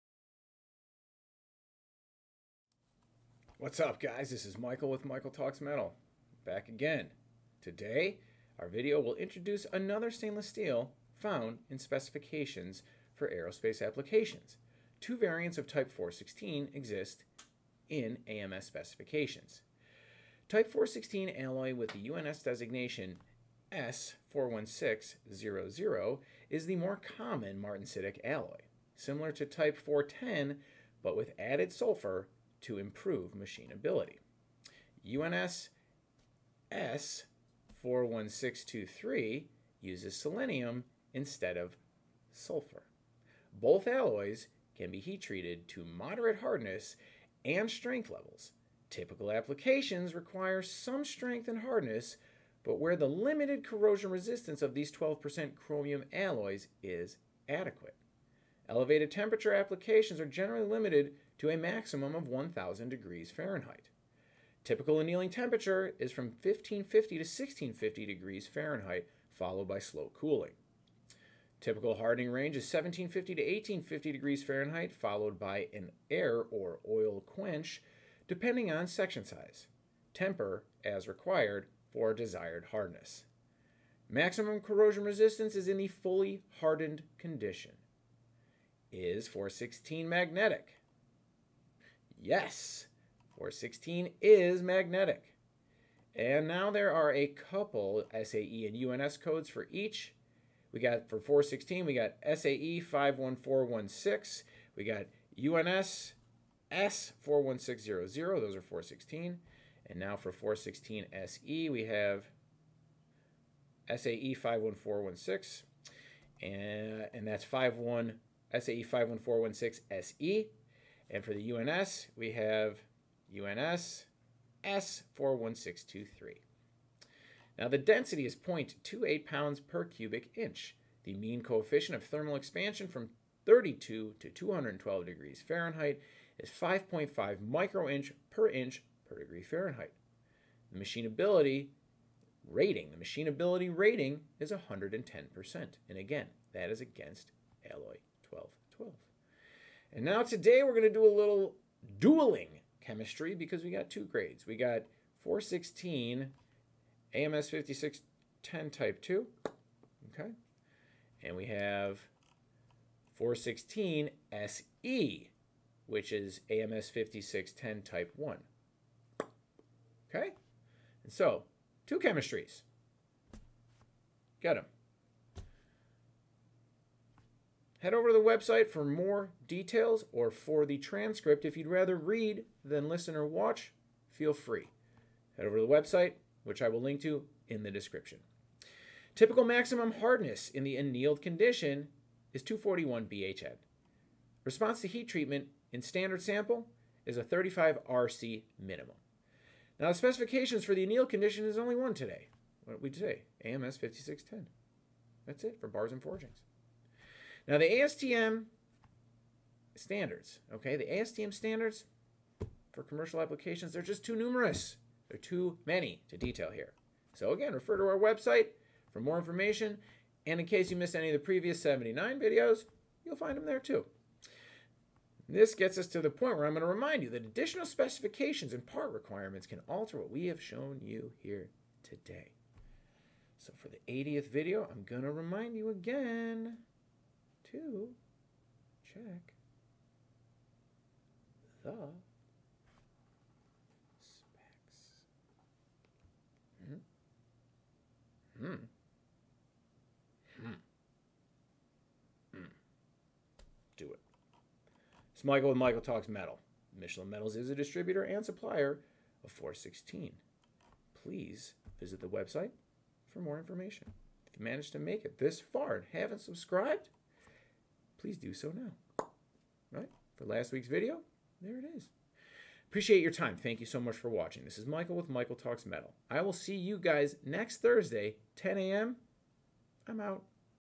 416 Stainless Steel AMS 5610 Type 1 and 2 416 Se
Today, our video will introduce another stainless steel found in specifications for aerospace applications. Two variants of type 416 exist in AMS Specifications. Full Transcript:

Type 416 alloy with the UNS designation S41600 is the more common martensitic alloy; similar to type 410 but with added sulfer to improve machinability. UNS S41623 uses selenium instead of sulfer. Both alloys can be heat treated to moderate hardness and strength levels. Typical applications require some strength and hardness but where the limited corrosion resistance of these 12% chromium alloys is adequate. Elevated temperature applications are generally limited to a maximum of 1000 deg F.

Typical annealing temperature is from 1550 to 1650 deg F followed by slow cooling.

Typical hardening range is 1750 to 1850 deg F followed by an air or oil quench depending on section size. Temper as required for desired hardness

Maximum corrosion resistance is in the fully hardened condition.

Is 416 magnetic? Yes, 416 is magnetic.